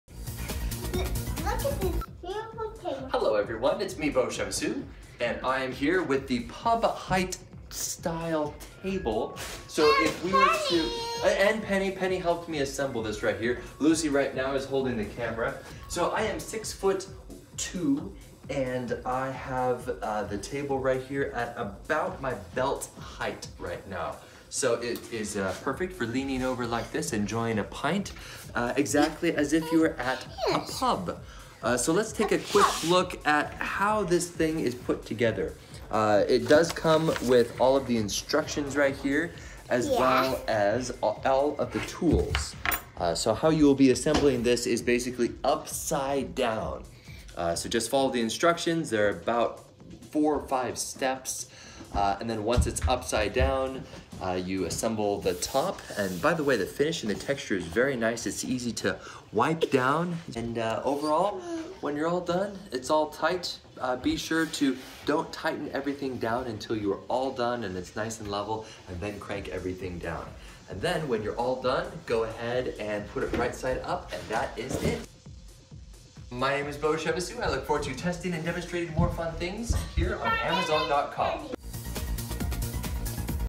★★★★★ Mr IRONSTONE Bar Table 47” Pub Dining Height Table - Review Bistro Table
Here's the link to this exact table on Amazon
Pros:
- Easy to put together.  (all the parts are numbered and match the instructions)
- Very sturdy
- Nice wood textures / easy to clean surface
- Perfect height for doing small tasks (e.g. sipping a drink, reading a quick magazine, etc)
- Arrived in perfect condition. I really wasn't sure if there would be dings and scratches.

Cons:
- Lots of packing material. *Not really a 'con' per se, but just be prepared they do NOT want anything to show up scratched. I was a little surprised.